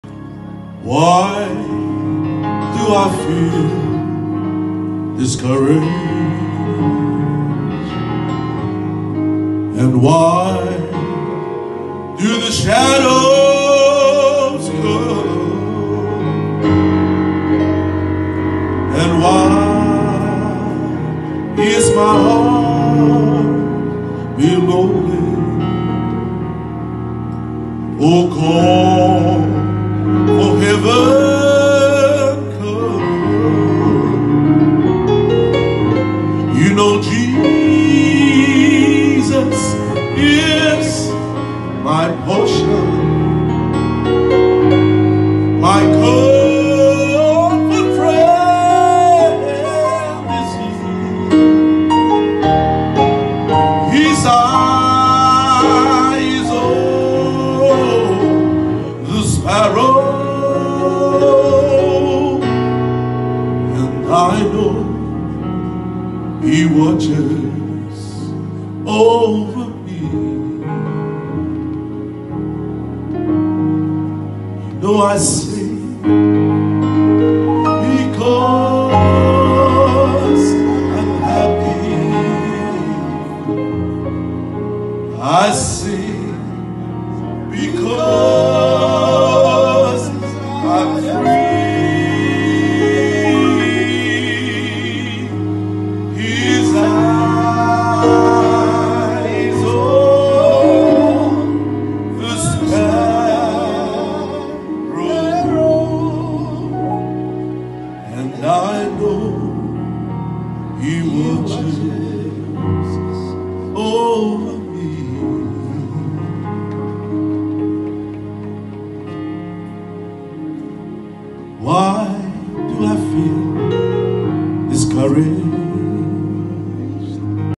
Pastor Kelemedi & Pastor Manu Lave
The brothers sing at the celebration of life for their father.